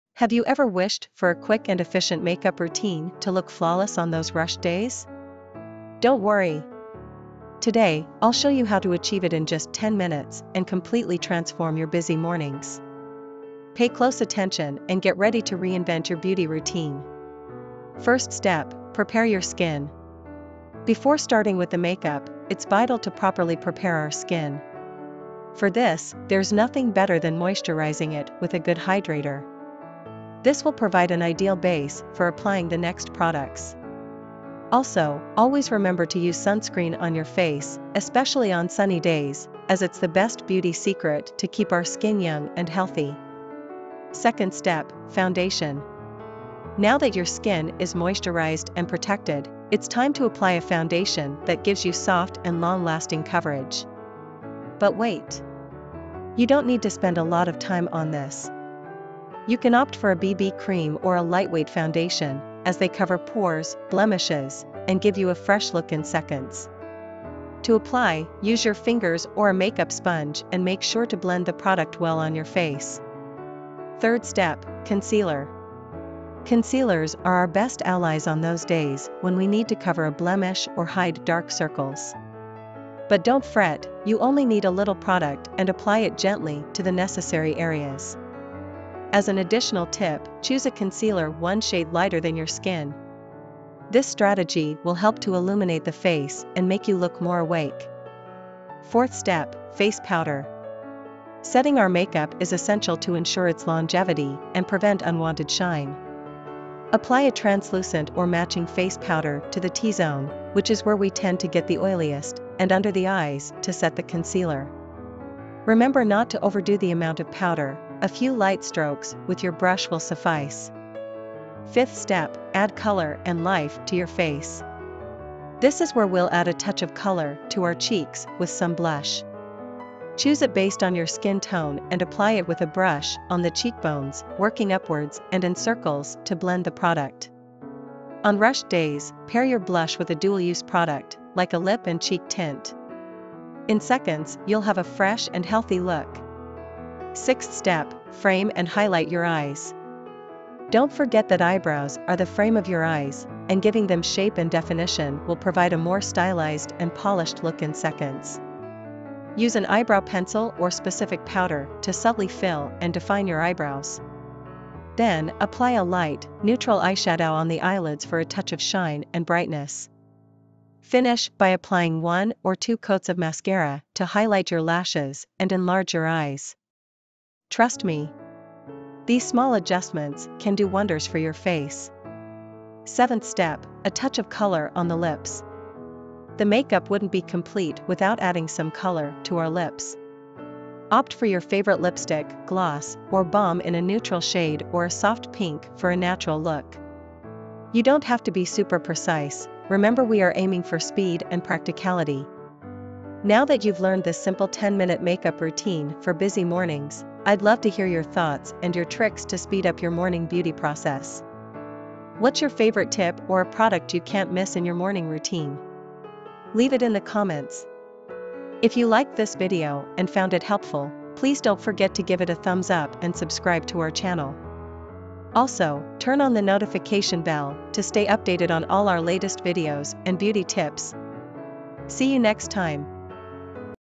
Beauty in 10 minutes Quick makeup routine for busy mornings
"Wake up late? Rushing out the door?⏰💄 Don't sacrifice your glow! Discover the ULTIMATE 10-minute beauty routine for those hectic mornings. Transform from bedhead to head-turner with these quick and foolproof tips! 💁‍♀️✨ Watch now to master the art of looking fabulous on-the-fly!"

In today's video, we're diving straight into a no-fuss, streamlined makeup routine perfect for those days when you're short on time but still want to look put-together. Whether you're a seasoned beauty guru or a makeup newbie, these simple steps are tailor-made for the modern woman on-the-go.

💡 Highlights:

The must-have products that'll speed up your routine.
Expert application techniques for maximum impact in minimal time.
Tips to ensure your quick makeup stays fresh all day.

Thank you for joining us on this beauty journey!